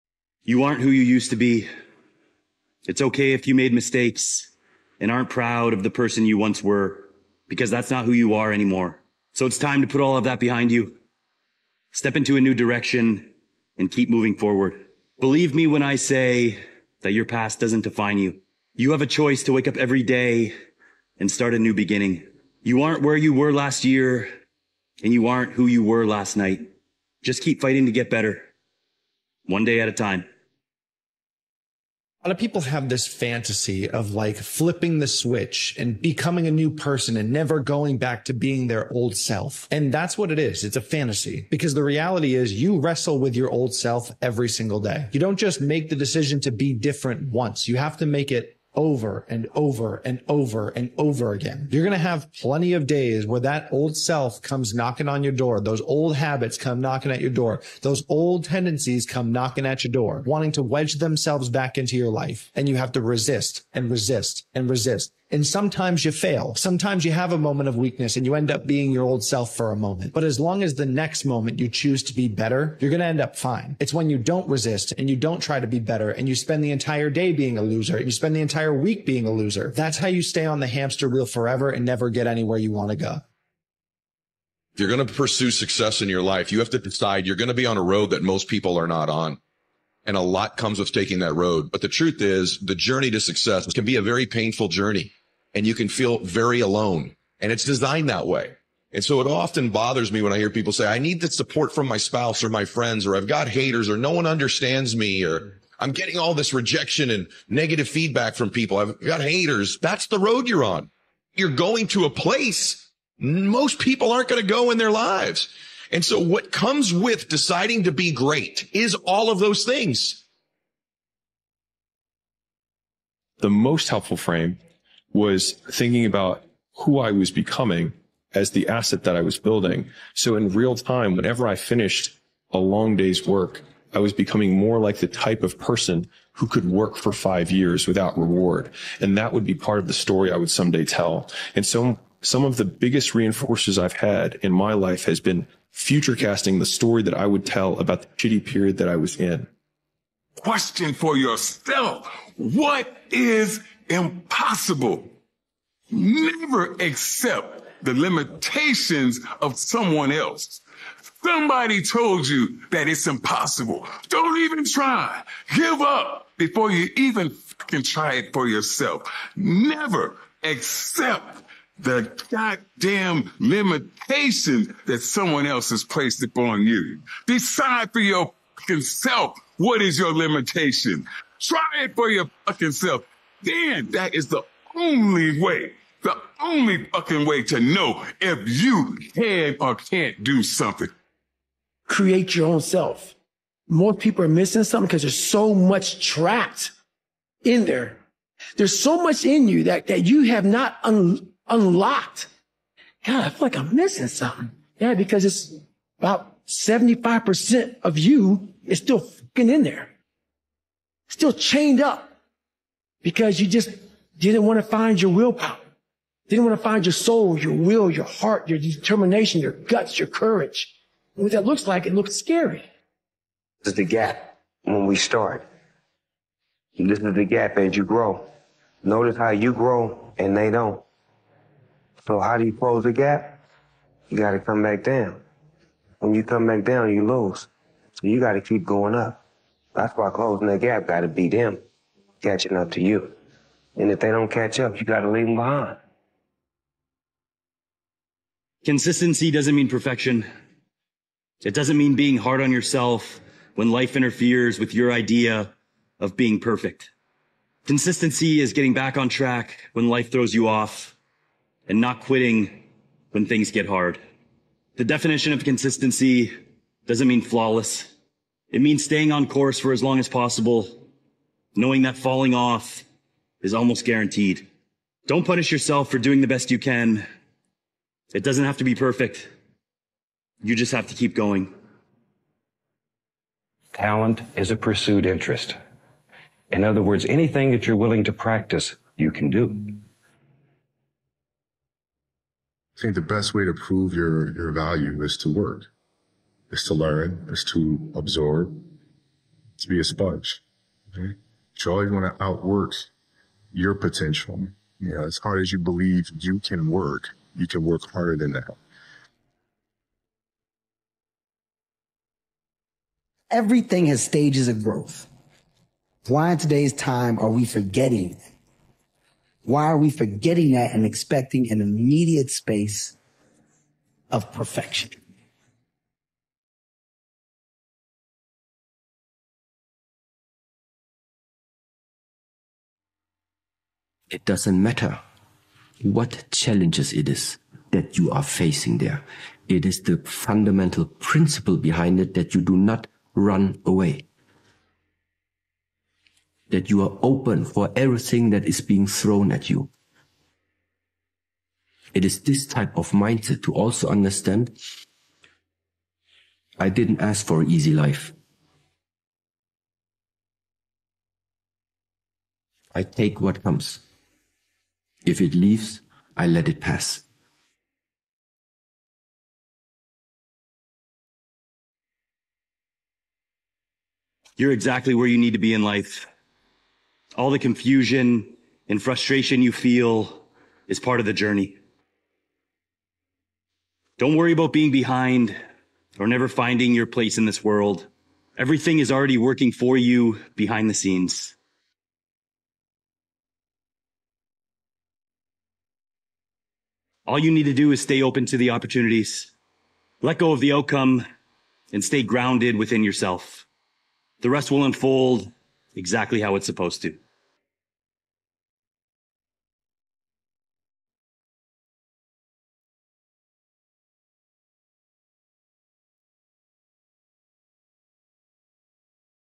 Take back your control and show them who you are!!! Best Motivation Experts Speech
You aren't who you used to be.  It's okay if you made mistakes, and aren't proud of the person you once were, because that's not who you are anymore.
So it's time to put all of that behind you, step into a new direction, and keep moving forward. Believe me when I say, that your past doesn't define you. You have a choice to wake up every day, and start a new beginning.

You aren't where you were last year, And you aren't who you were last night. Just keep fighting to get better.  One day at a time.
A lot of people have this fantasy of like, flipping the switch and becoming a new person and never going back to being their old self. And that's what it is. It's a fantasy.